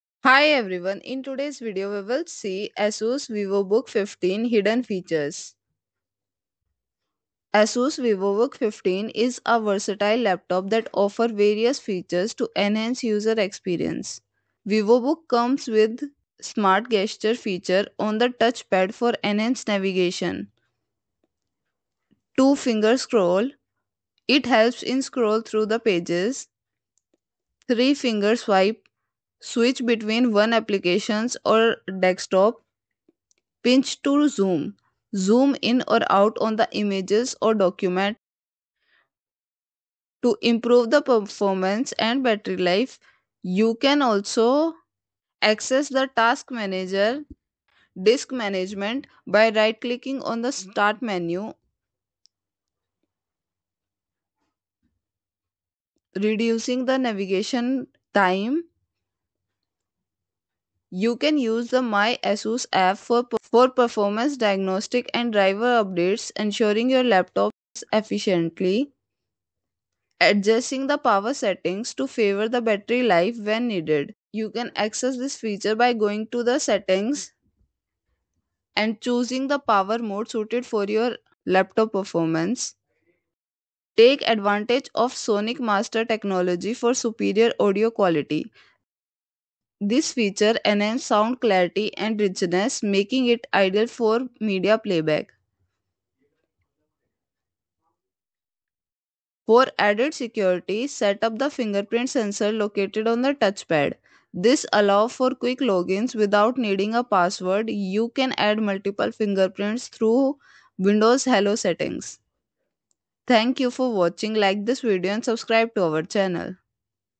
ASUS VivoBook 15 Hidden Features
In This Video, We Explore The Hidden Features Of The ASUS VivoBook 15 That You May Not Be Aware Of. We Uncover Tips And Tricks That Can Elevate Your Daily Tasks And Improve Your Overall Experience With This Versatile Laptop. These Features Will Help You Unlock The Full Potential Of Your Device.